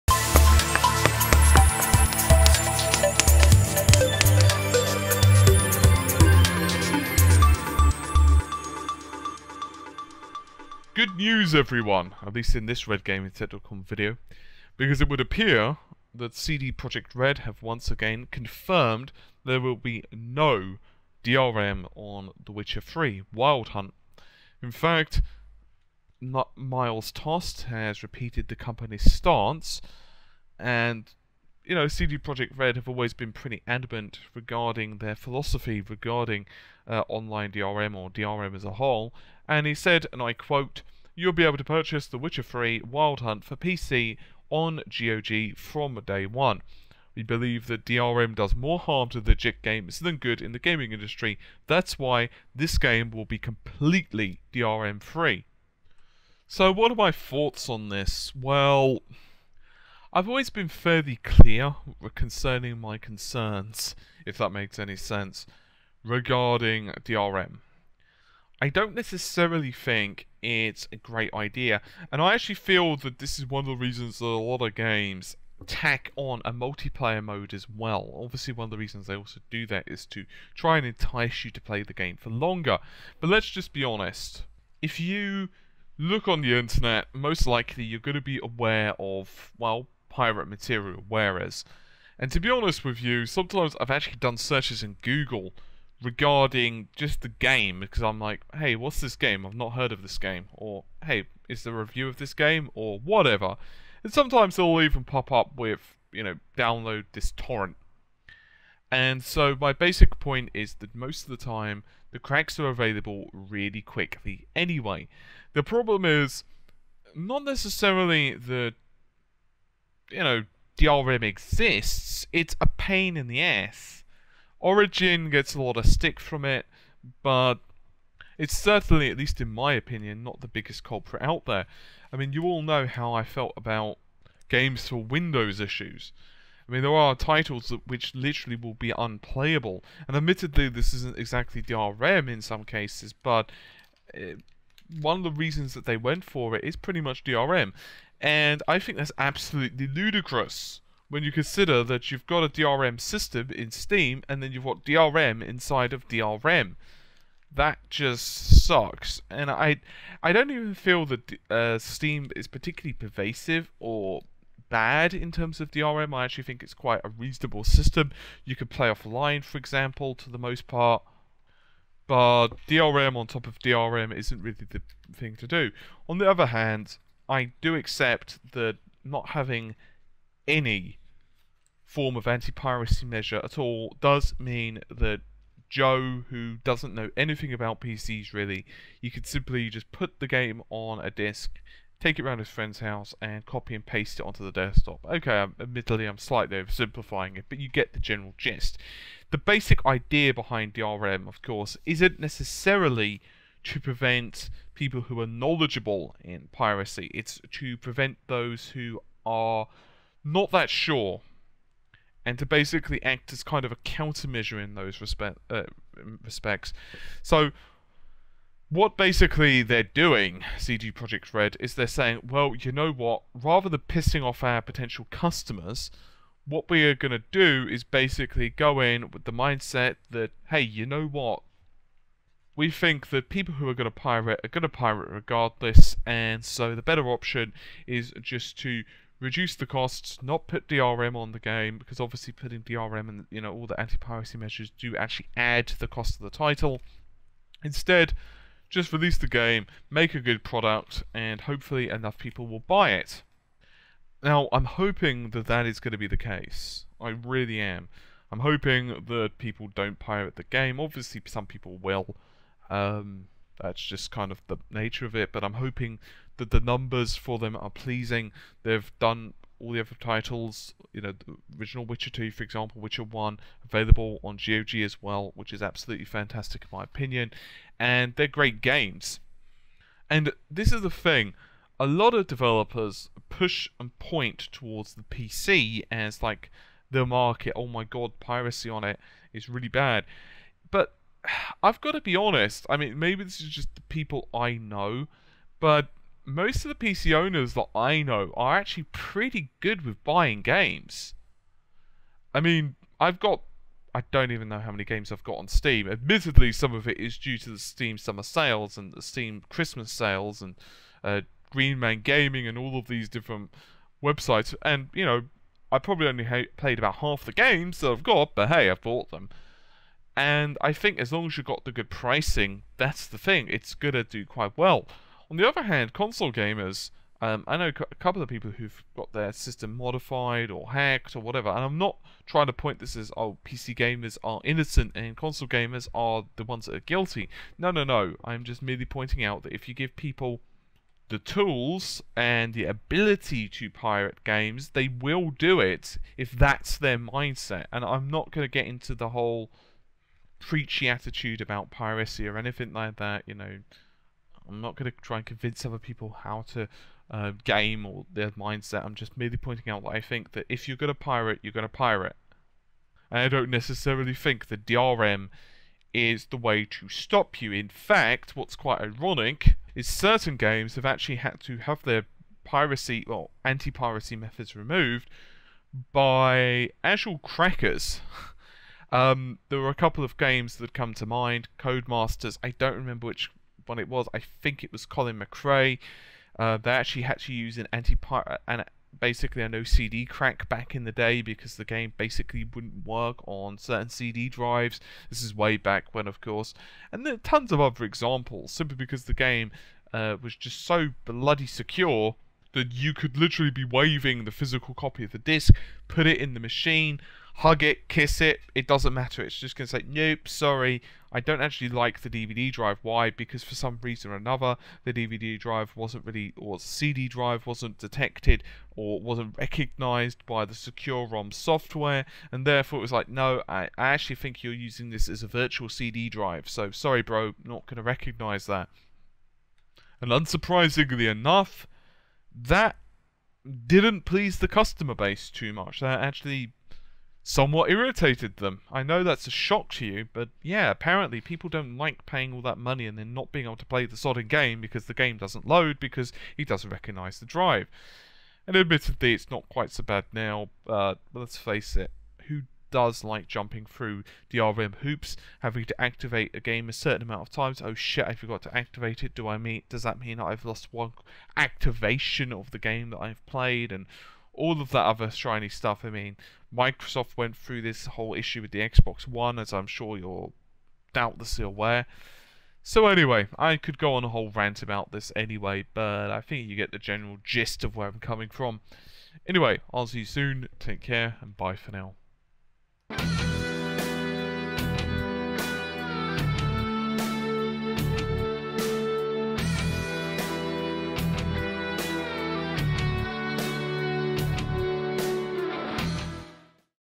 Witcher 3 Contains No DRM Confirms CD Projekt Red - Other Devs Please Take Note
So, CD Project RED have gone on record (again) and pointed out that they'll not be enforcing DRM (Digital Rights Management) with the upcoming Witcher 3. They'd applied a similar stance to the prior games in the Witcher series as doubtlessly many PC gamers will be all to aware.

The bigger questions is why can't more PC game studios follow the same path?